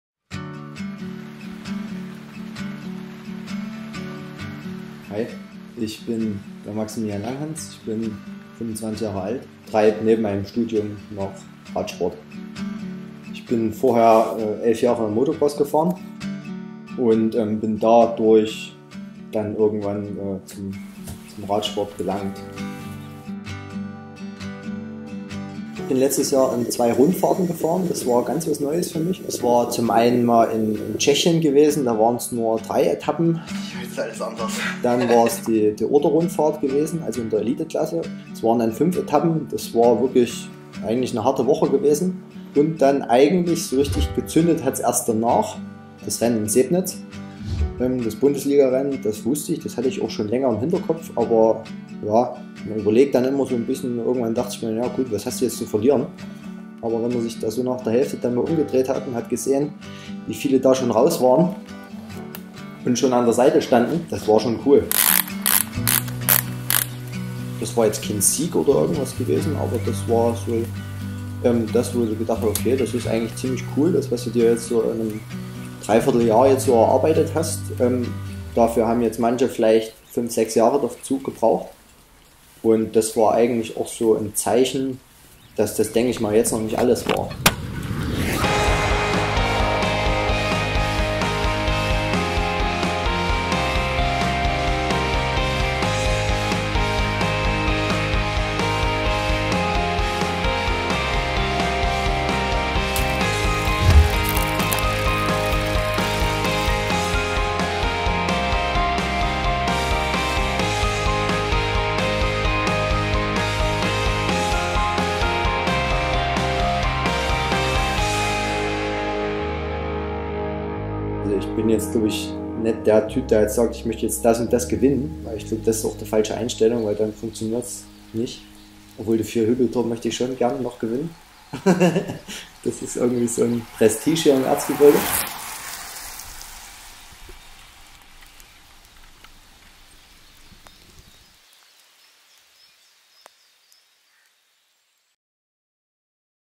Ein Tag bei Maximilian Langhans
Seine Heimat ist das Erzgebirge. Und dort trainiert Maximilian Langhans fast täglich um seine Träume in die Tat umzusetzen.
Wir haben ihn einen Tag, bei fast typischen Gebirgswetter, begleiten dürfen.